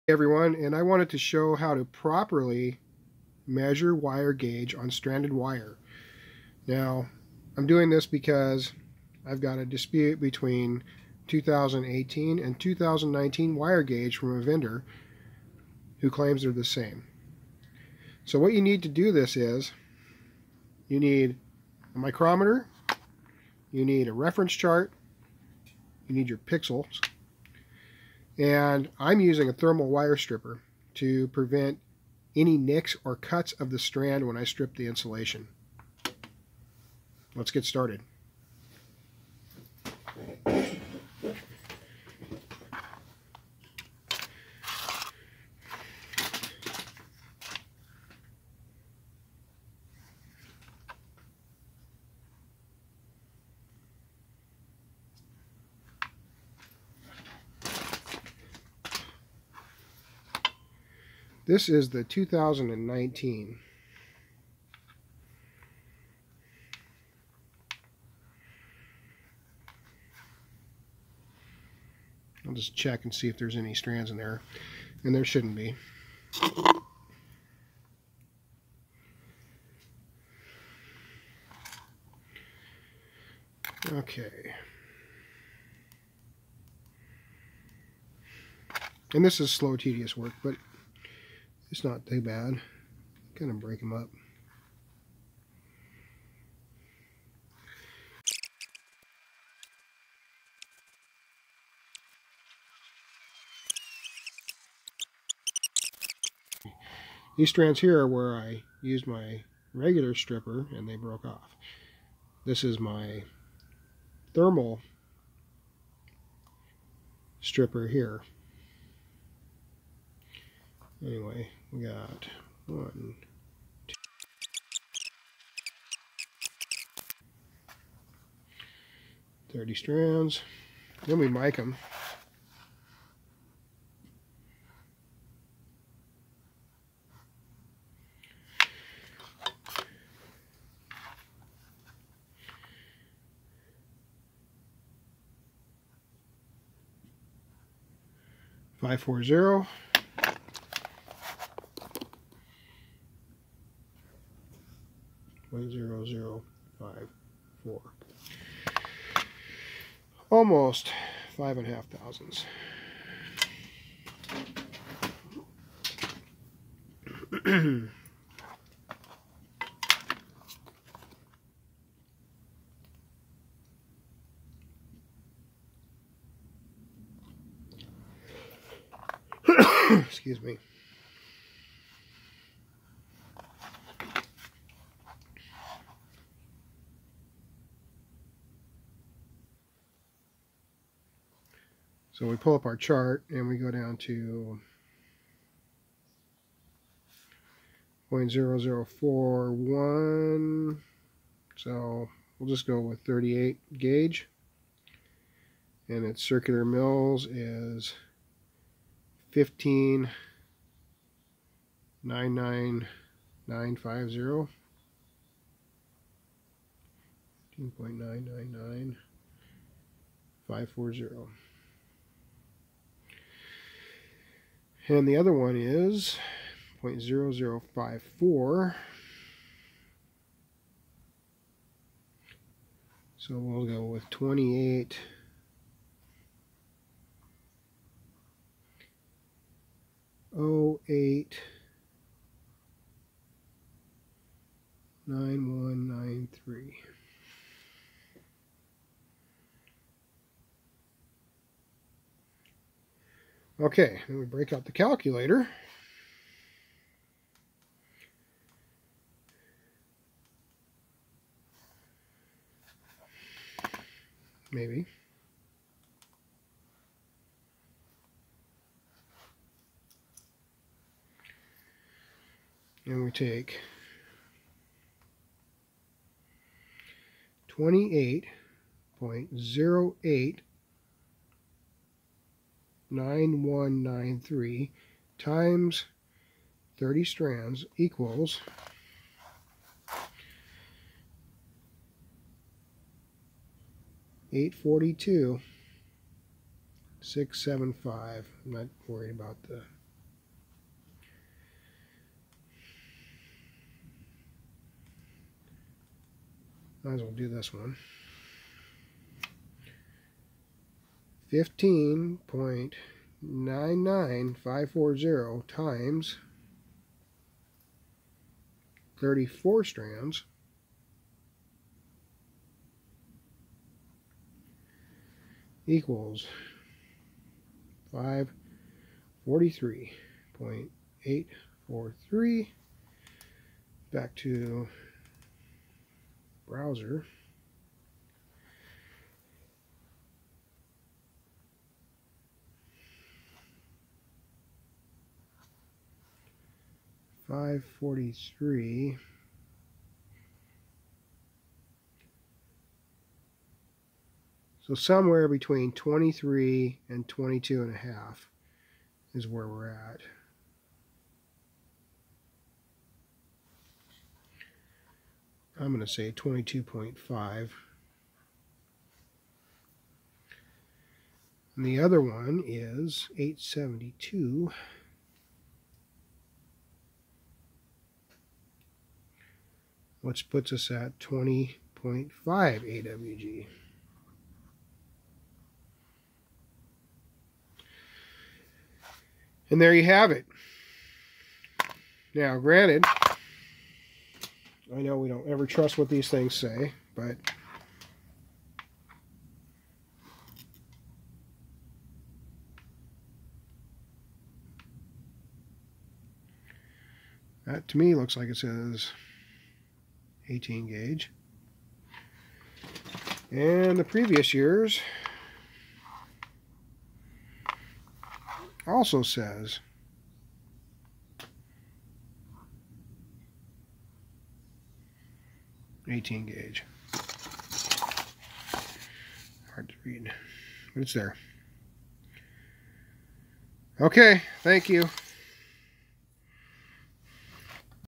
Calculating stranded wire gauge
Here I show how to properly strip, measure, and then calculate the wire size of stranded wire.